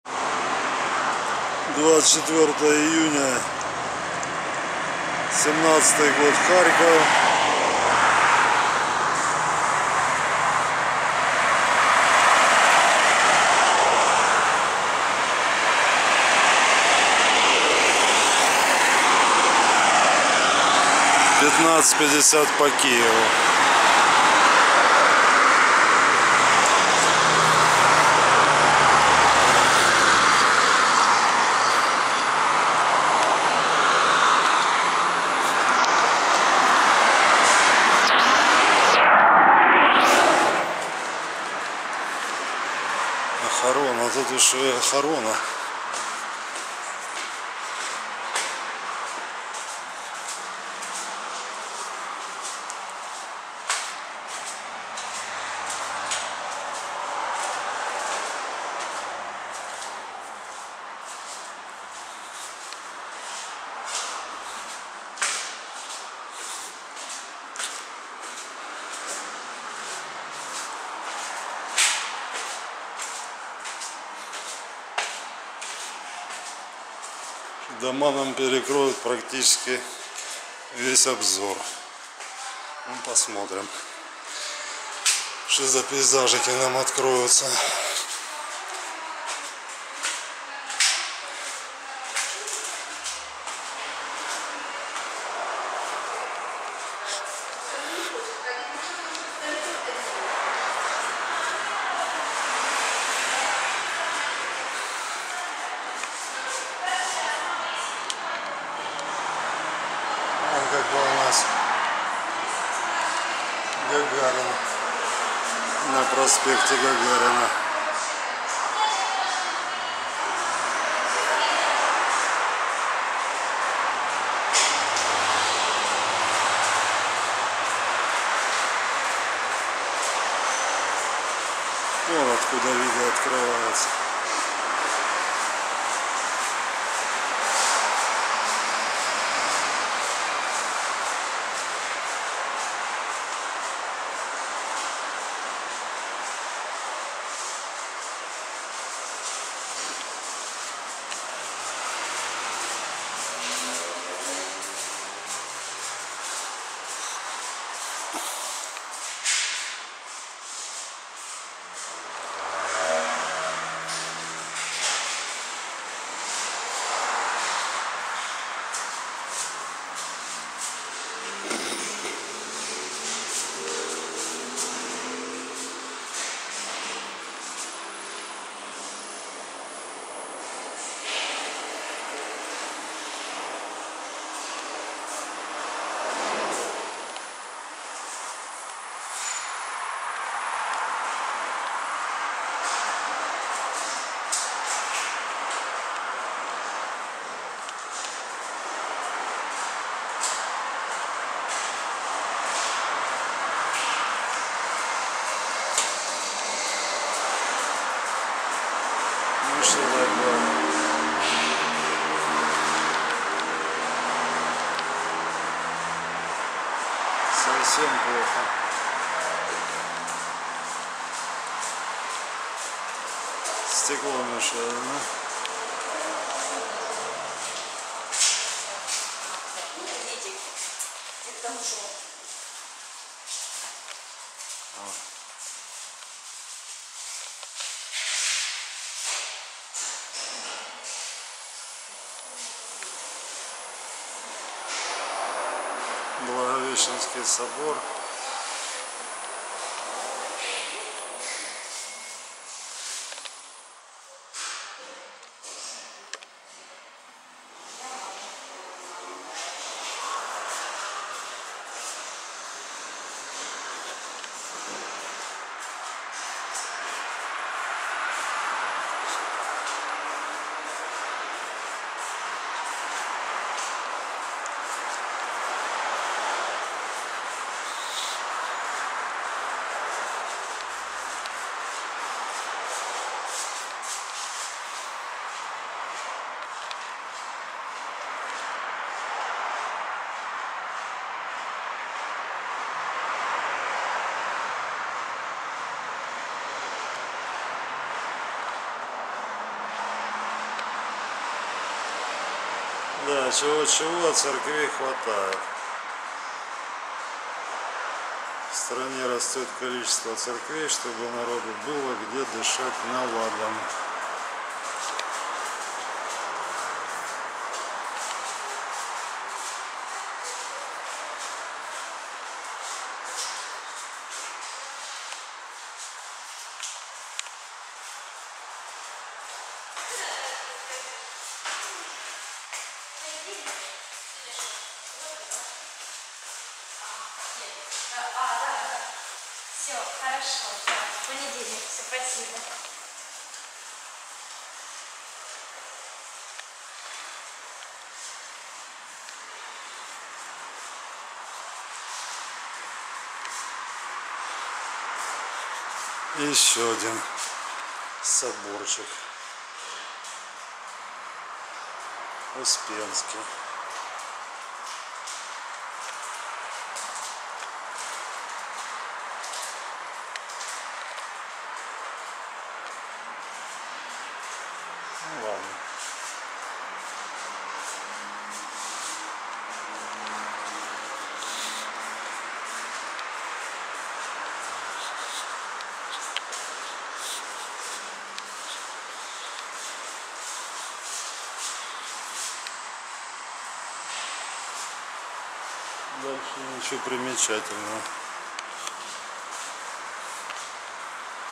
Надземный переход на проспекте Гагарина в Харькове, 24 июня 2017
Пешеходный мост в Харькове на проспекте Гагарина.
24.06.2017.